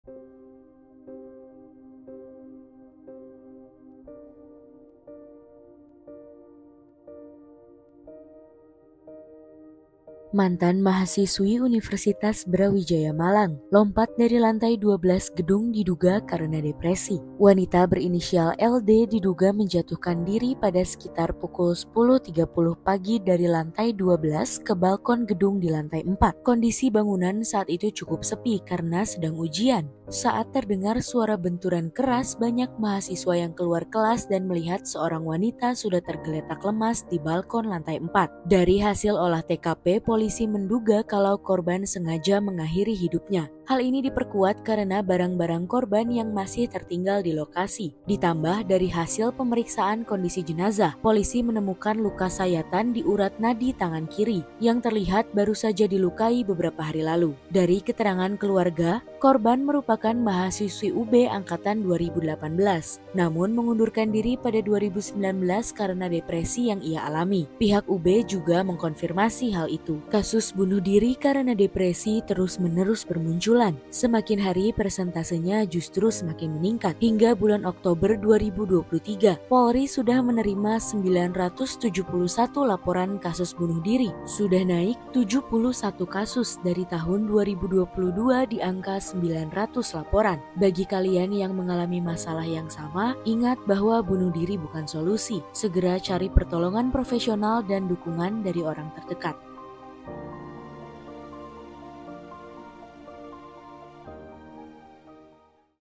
Lagi, Orang Muda Bunuh Diri karena Depresi
Seorang mantan mahasiswi Universitas Brawijaya Malang, mengakhiri hidupnya dengan meloncat dari lantai 12 gedung kampus. Ia ditemukan tergeletak lemas sekitar pukul 10.30 ketika mahasiswa lain sedang melaksanakan ujian akhir.
Video Editor: Imawati Tia
Produser: Nibras Nada Nailufar